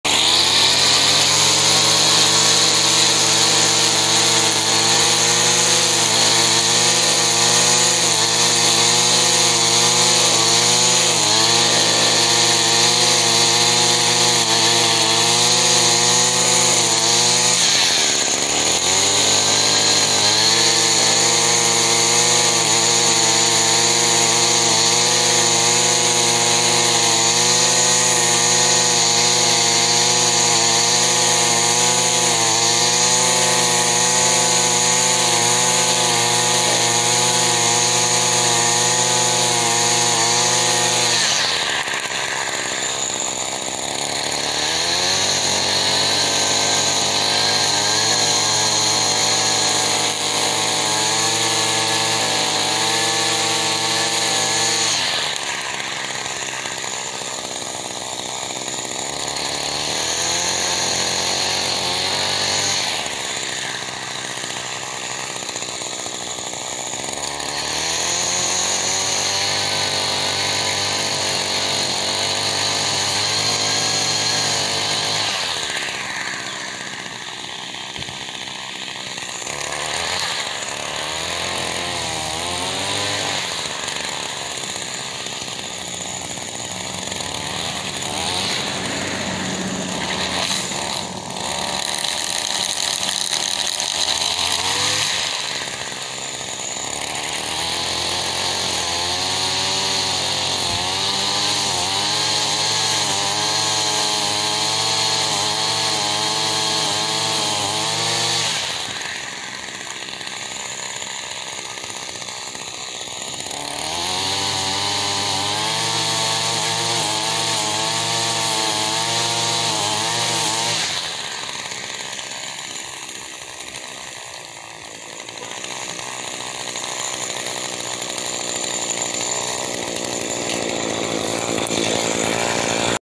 Been Nearly 2 Years Since I Recorded Myself One Handed Holding Weed Wacker 12 Pounds Edging Sidewalk
Been Nearly 2 Years SinceTheLastTime I Recorded Myself OneHanded ( OnlyMyRightArm ) Edging and ( That was August24th2020 - Not My Last Doing One Handed)
and This EchoWeedWacker Almost6Feet InLength That Weigh 12Pounds Full Mix Gas & and is Still MyHeaviest ToHandle LongestHold Edging Sidewalk Perfectly

ThisIsNot The Longest One Handed Hold I was doing some before the recording and this sidewalk part was the last part before continuing doing the whole yard.